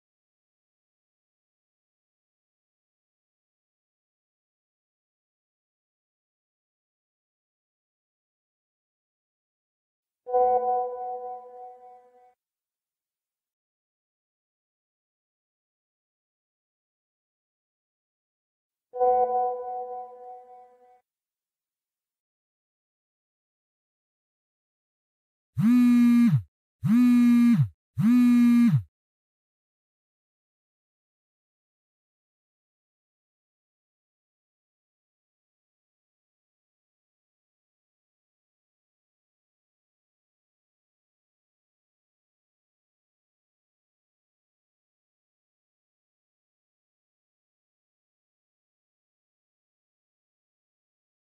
Messages people get vs messages I get. Normal people vs me.
other people typically get messages from a special person, meanwhile I get messages telling me to send or forward this message to a bunch of friends of mine, or something terrible will happen 😅